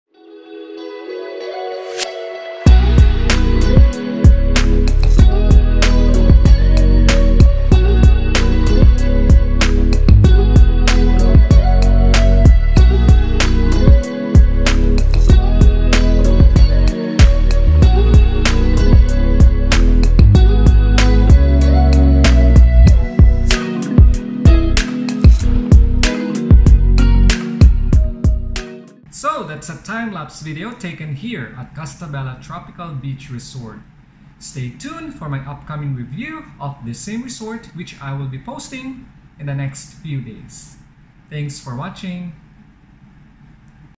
Time-lapse Video taken Costabella Tropical Beach Hotel, Mactan, Cebu Beach Resort
I uploaded this video under creative commons, you are free to use without attribution required. Here's the link to the raw file/video.

A timelapse video of Costabella Tropical Beach Hotel, located in Mactan Island, Buyong Rd, Maribago, Lapu-Lapu City, 6015 Cebu

This video was taken October 11, 2021. I am currently making my review about this resort for the duration of my stay; I will be uploading it as soon as I am finished editing, within the next few days.